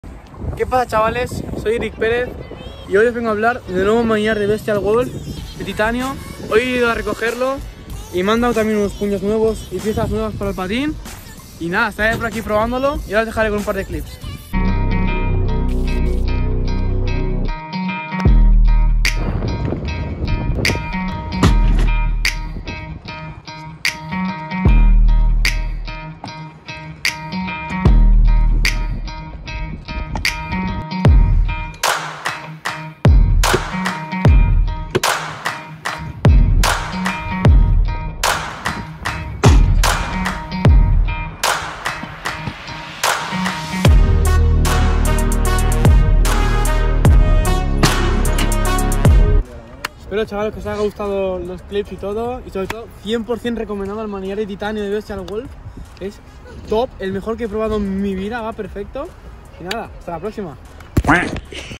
ERIC PEREZ - TITANIUM HANDLEBAR TEST 🐺🔥🛴
En este video, el rider Eric Perez hará un testing del nuevo manillar de titanio Bestial Wolf.

Este manillar es una auténtica bestia, fabricado con materiales de alta calidad que garantizan su durabilidad y resistencia ante cualquier tipo de truco o caída. Además, su diseño moderno y elegante lo convierten en el accesorio perfecto para darle un toque único a tu scooter.

Eric nos mostrará todas las características y ventajas que este manillar tiene para ofrecer, desde su ligereza hasta su capacidad de absorción de impactos. También nos dará su opinión personal sobre cómo se siente al usarlo y nos mostrará algunos de sus trucos favoritos con este nuevo manillar.

Así que si eres un amante del mundo del scooter y te interesa estar siempre a la vanguardia en cuanto a accesorios y tecnología, ¡no te pierdas este video! Prepárate para descubrir todas las ventajas del nuevo manillar para scooter de titanio Bestial Wolf de la mano de uno de los mejores riders del momento. ¡Vamos a verlo!